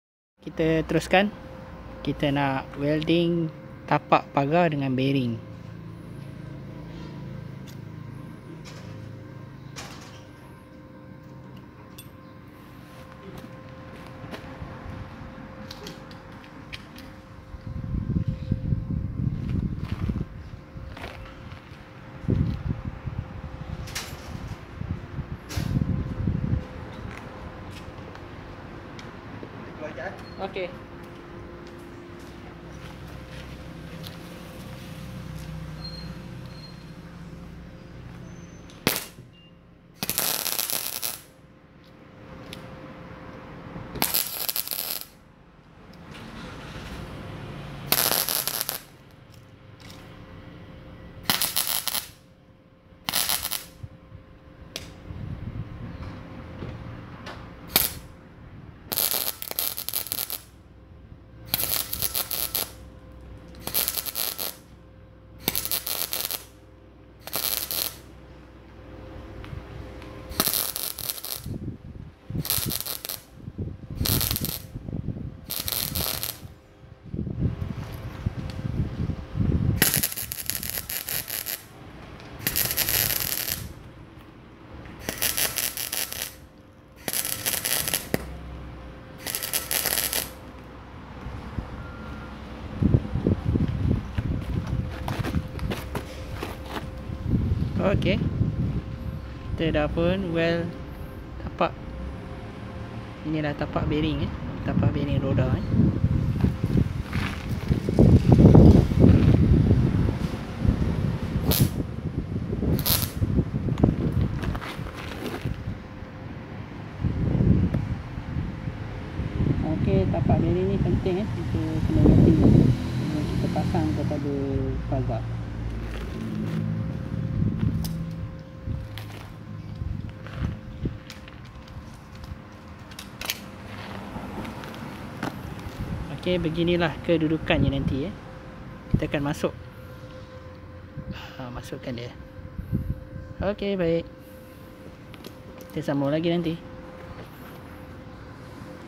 Baiki roda pagar Bahagian 2. No. Tel. Kami: 0192462024
Bahagian ini menunjukkan cara memasang tapak bearing roda